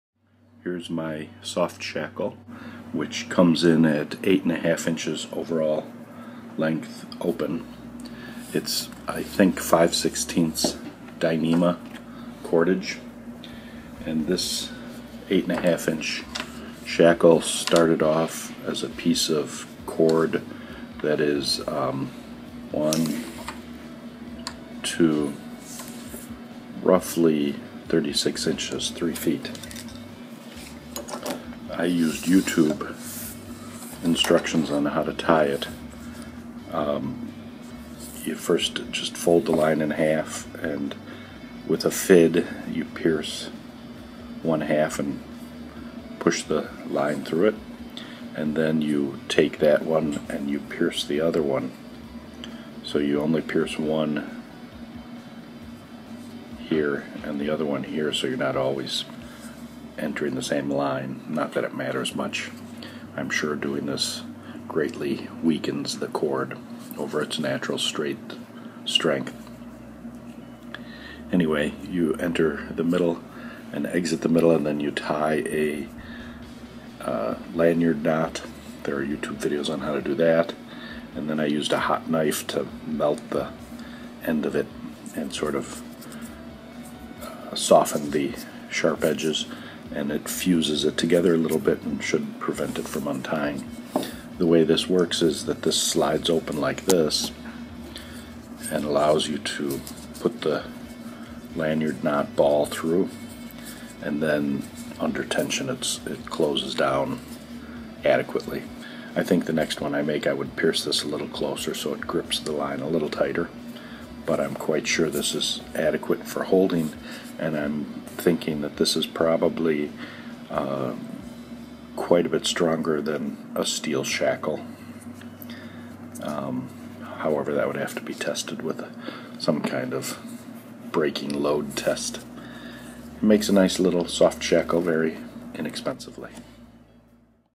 DYI Soft Shackle
This is an examination of the soft shackle I made for non-critical sailing applications.

This is not instructions on how to make one.

Comparison of s.s. construction methods for maximum strength.